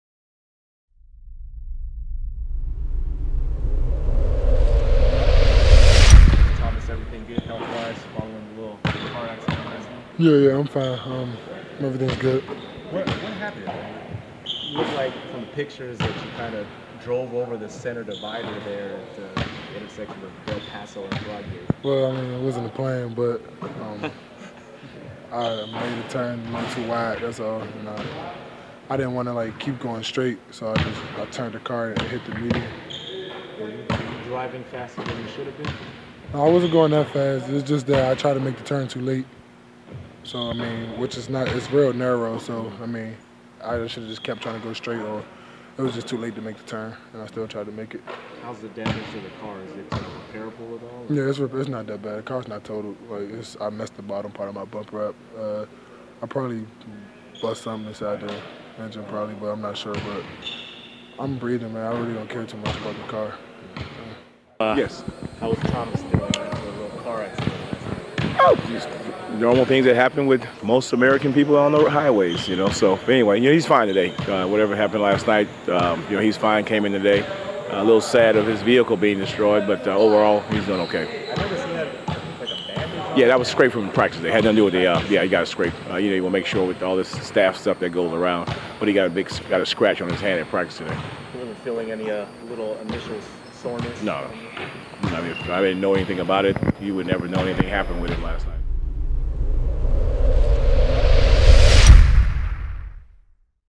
Thomas Robinson on minor car accident
The Kings rookie big man talks about a minor car accident he sustained after the Kings win over the Cavs.